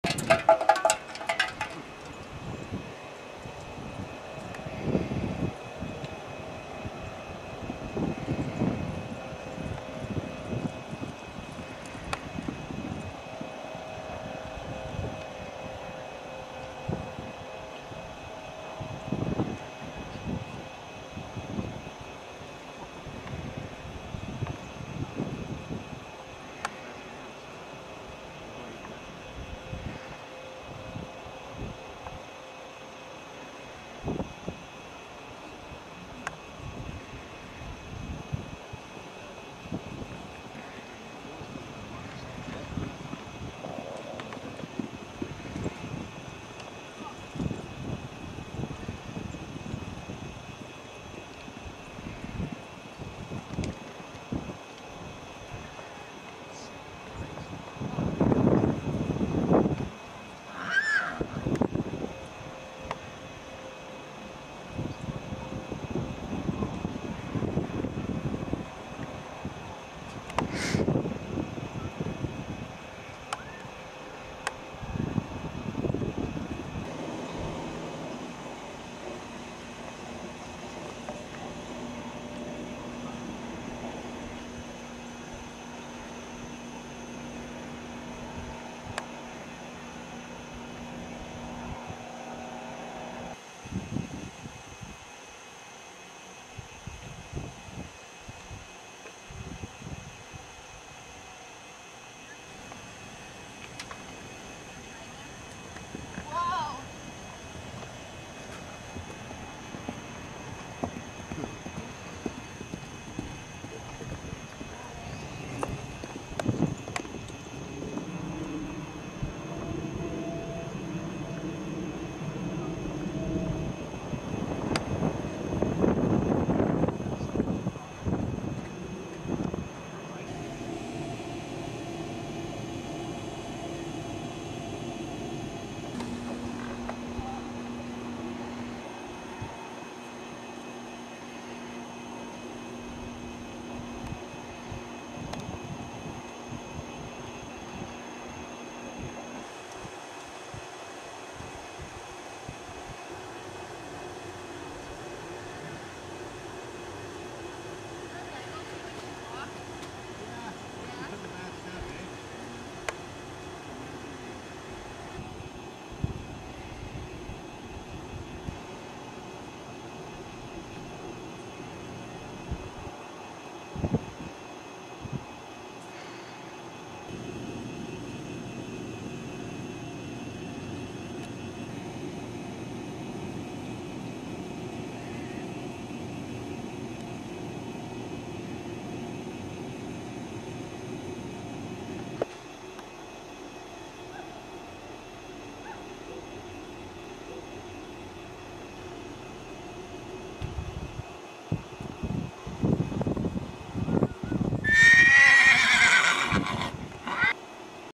VTS_01_1.VOB
Horse King Joe25 yrs,running with our 5 month foal. He thinks he is her father but Cat Call is really the sire.He does a good job keeping up with Khloe.